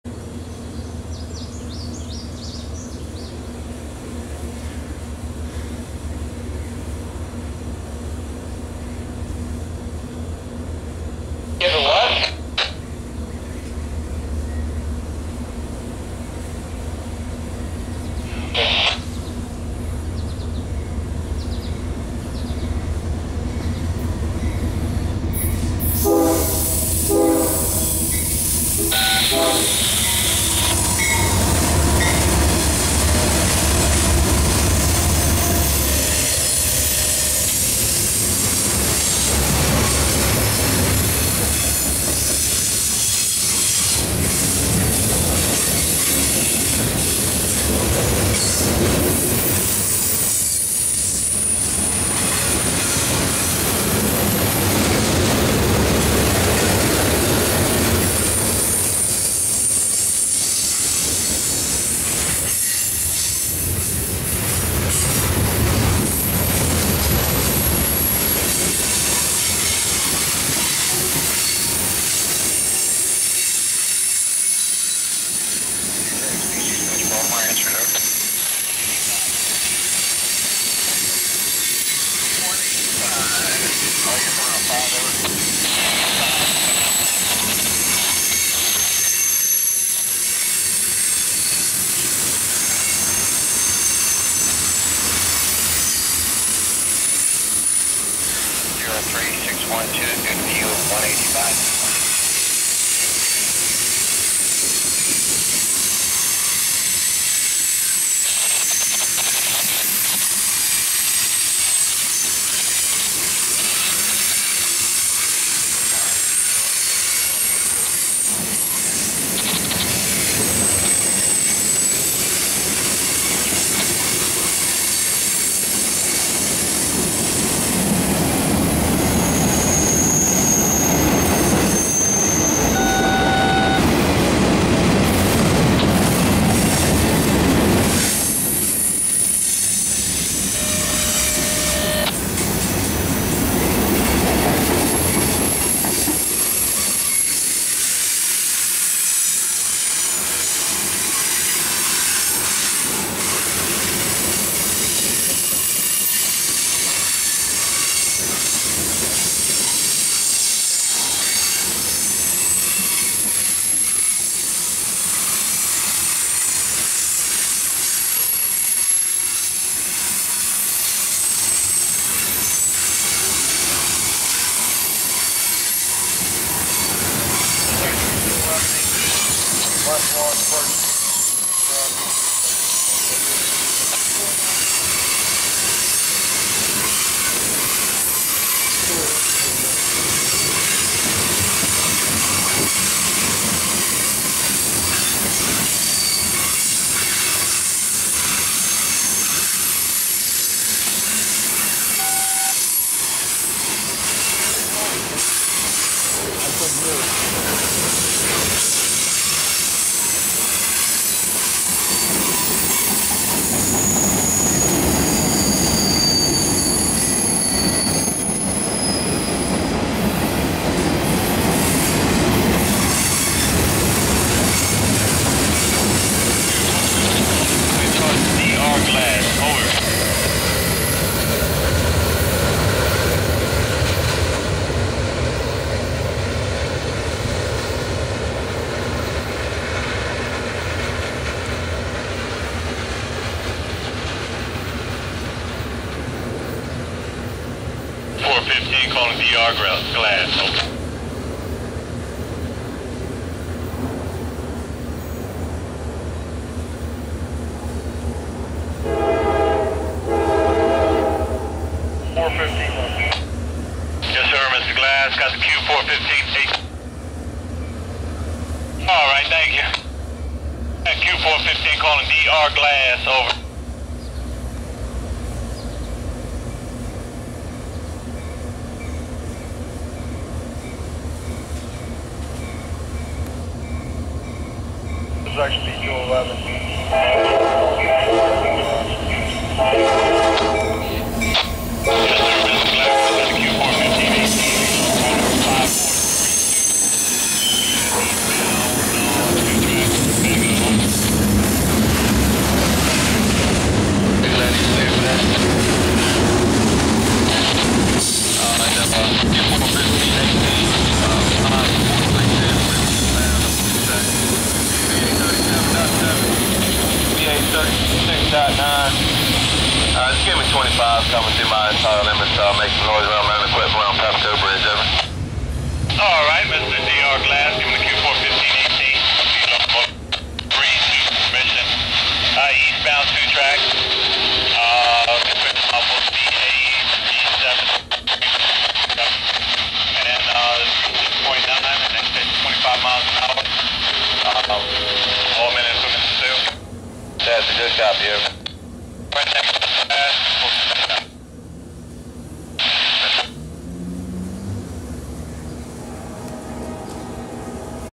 CSX Train Meet W/ Western MD Sticker Heritage Unit
Eastbound CSX Train Q415 With Western MD Sticker Heritage Unit Meets Westbound CSX Train In Point Of Rocks, Frederick County, Maryland. 7 18 17